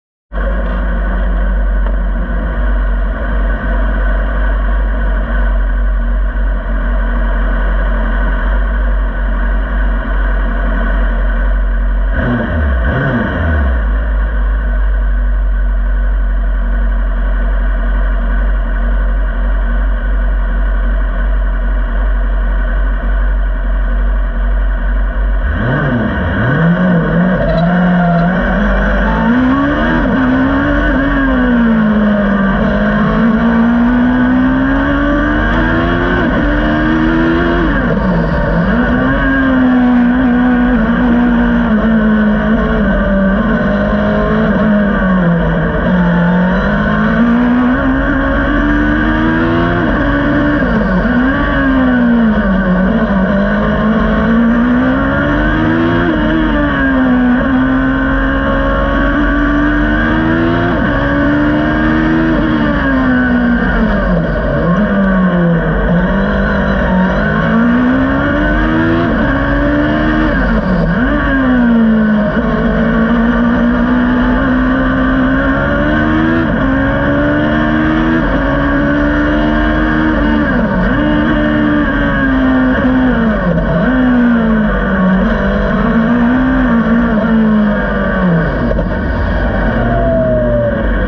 Harewood Hill climb, Caterham Hayabusa 59.38
Best run of the weekend of 59.38s.  A bit off pb.  9th or if field of 135...